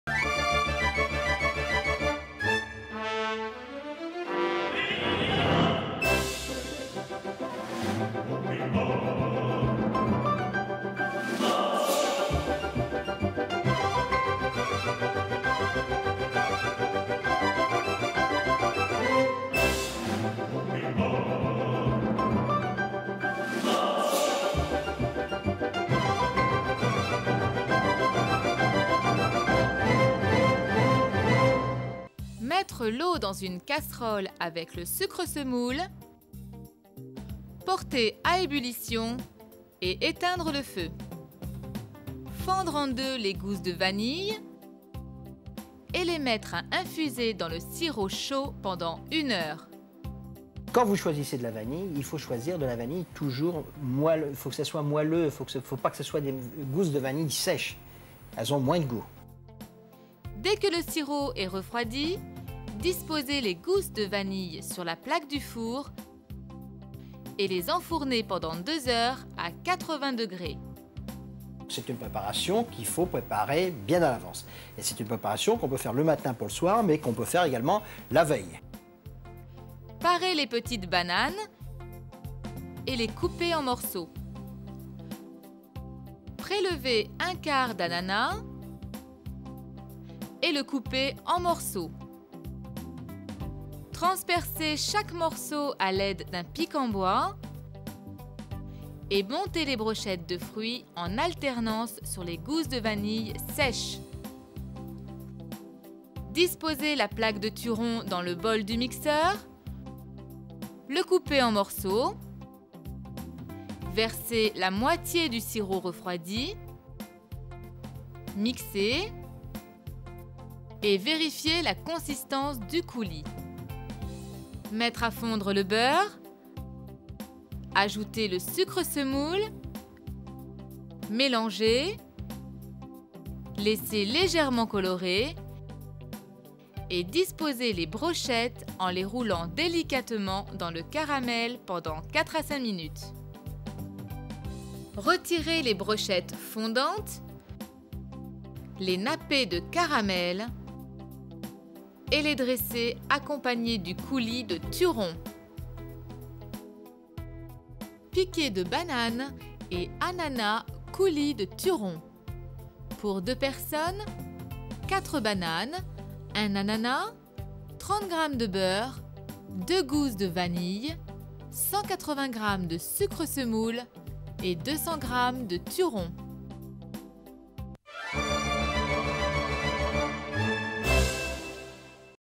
[Recette] Piqué de Bananes et Ananas sur Gousse de Vanille - Chefs Jacques et Laurent Pourcel
[Recette] Piqué de Bananes et Ananas sur une Gousse de Vanille Coulis de Turon
▼▼ Ci-Dessous la RECETTE et les INGRÉDIENTS ▼▼

Recette pour 2 Personnes

Ingrédients :
4 bananes freysinette
1 quartier d’ananas
2 gousses de vanille
1 plaque de Turon de 200 g
180 g de sucre semoule
30 g de beurre

Recette :
1) Au préalable:
Fendre en deux 2 gousses de vanille.

Un conseil:
A l’achat, choisir des gousses de vanille moelleuses, pas sèchent.

Verser 1/4 de litre d’eau dans une casserole, puis ajouter 150 g de sucre semoule et porter à ébullition.

Après une bonne ébullition, retirer le sirop du feu, y ajouter les gousses de vanille fendues et les laisser infuser pendant 1 heure.

2) Préchauffer le four à 80°C.

Les gousses de vanille ont infusé 1 heure dans le sirop.
Les égoutter, puis les disposer sur une plaque allant au four, et les glisser dans un four préchauffé à 80°C pendant 2 heures.
Il est possible de préparer ces gousses de vanille quelques heures avant, même quelques jours à l’avance les conserver alors dans une boite hermétique.

2 heures après, les gousses sont bien sèchent, elles sont dures, les réserver.

3) Retirer l’écorce d’un quartier d’ananas, éliminer la partie du cœur qui est dure.
Puis fendre ce quartier en deux, couper les deux morceaux en tranches assez épaisses afin d’avoir en tout 12 morceaux d’ananas.

Peler 4 petites bananes freysinette, parer les pointes, puis tailler ces bananes en 3 morceaux.

4) Avec un pic en bois, transpercer le centre de chaque morceau de fruit.

Puis monter 4 brochettes:
Piquer sur chaque demi gousse de vanille, en les intercalant, 3 morceaux de banane et 3 morceaux d’ananas.
Réserver les brochettes au réfrigérateur.

5) Mettre dans le bol d’un mixeur 1 plaque de Turon de 200 g coupé en morceaux, ajouter la moitié du sirop, et mixer.

6) Faire fondre 30 g de beurre dans une poêle sans coloration, ajouter 30 g de sucre semoule, mélanger et amener à coloration afin d’obtenir un caramel de couleur blonde.
Puis ajouter les brochettes sur feu doux, les poêler, les rouler dans le caramel délicatement, compter 4 à 5 minutes de cuisson.
Les fruits doivent être fondants.

Dresser 2 brochettes par personne dans des assiettes de service, les napper d’un petit peu de caramel, puis du coulis de Turon.